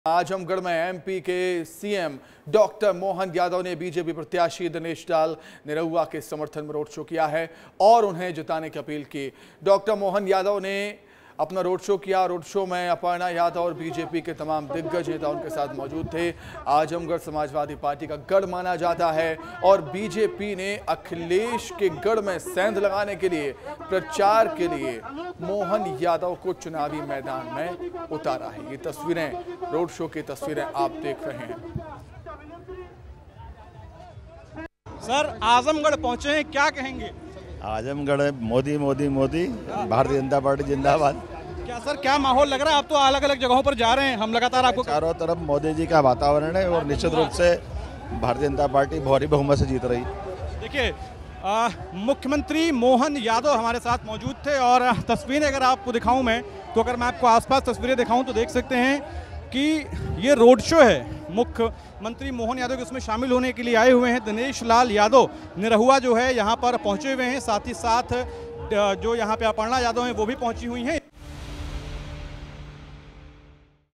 Exclusive : UP के Azamgarh में Road Show करने पहुंचे MP CM Mohan Yadav से ख़ास बातचीत | Vistaar News
आजमगढ़ में एमपी के सीएम डॉक्टर मोहन यादव ने बीजेपी प्रत्याशी दिनेश लाल निरहुआ के समर्थन में रोड शो किया. और उन्हें जिताने की अपील की. मोहन यादव के साथ रोड शो में अपर्णा यादव और बीजेपी के तमाम दिग्गज मौजूद थे. आजमगढ़ समाजवादी पार्टी का गढ़ माना जाता है. और बीजेपी ने अखिलेश के गढ़ में सेंध लगाने के लिए प्रचार में मोहन यादव को उतारा है.

Join the roadshow in Azamgarh led by MP CM Dr. Mohan Yadav in favor of BJP candidate Dinesh Lal Nirahua, with a call to secure victory. Aparna Yadav and prominent BJP figures were part of the event. Azamgarh, traditionally a Samajwadi Party fortress, is now a target for BJP to challenge Akhilesh's dominance, with Mohan Yadav spearheading the efforts.